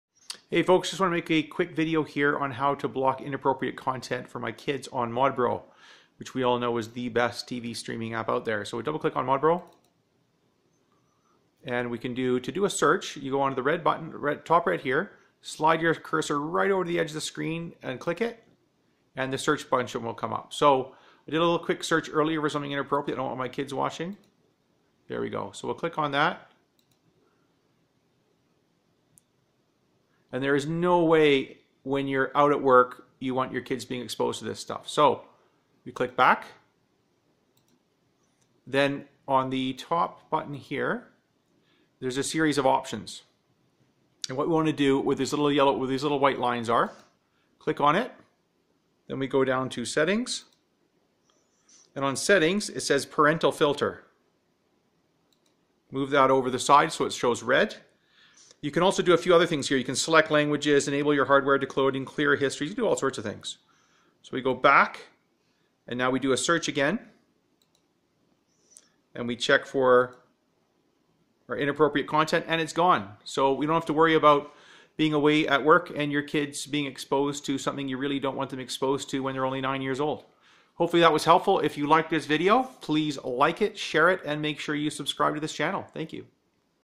Block adult content in Mobdro and do a search
Here is how to quickly block adult content/pornography on Mobdro and do a search